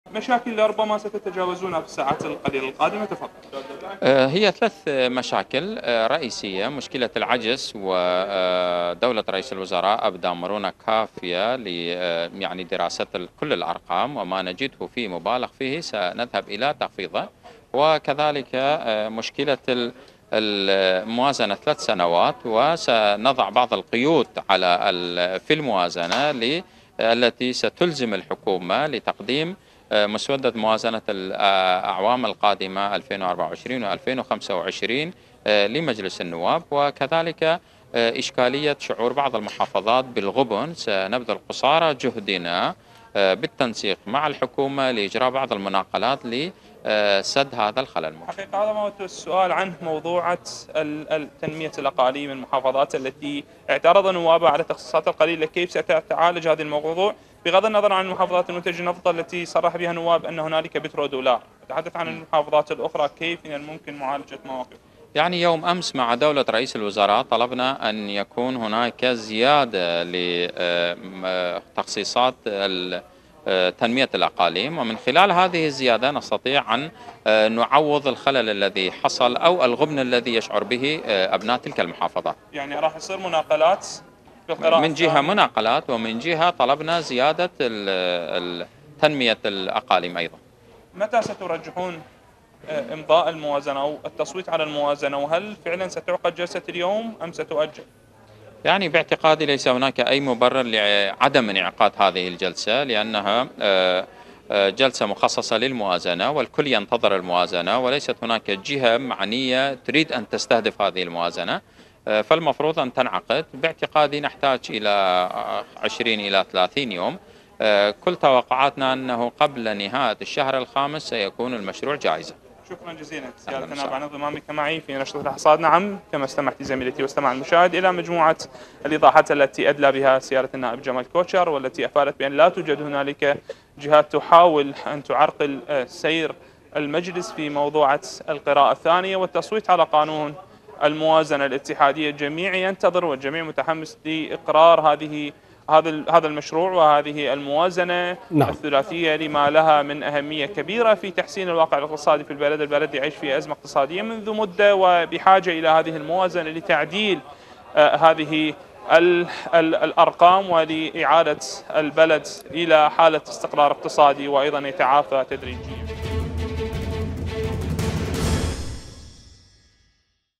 جمال كوجر يكشف أبرز المشاكل في الموازنة الاتحادية
النائب جمال كوجر يكشف أبرز المشاكل في الموازنة الاتحادية
موقع قناة التغيير الفضائية  الالكتروني